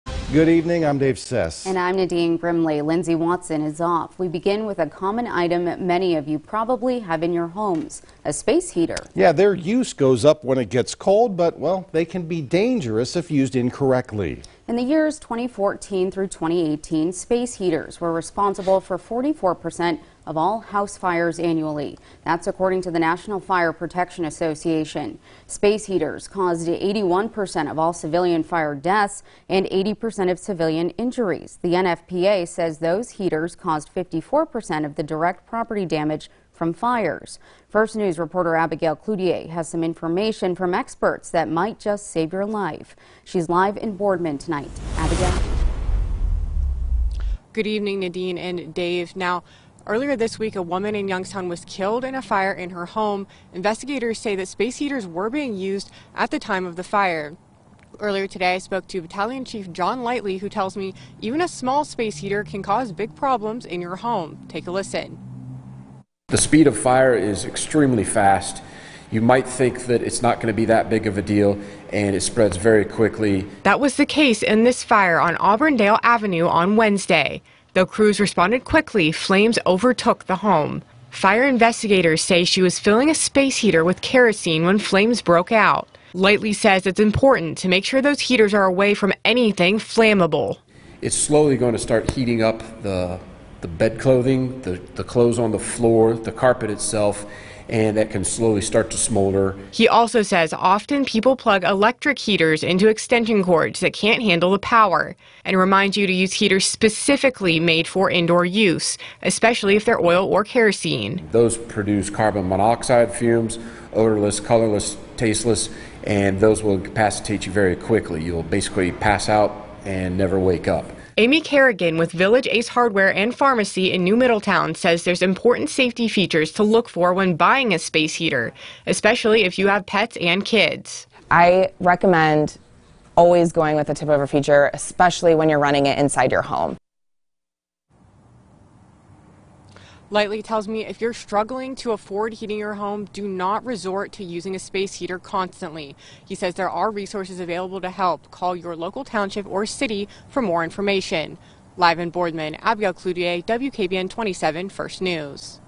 Space heaters: Staying safe while staying warm
Earlier this week, a woman was killed in a fire at her home on Auburndale. Investigators say she was filling a space heater with kerosene when it happened.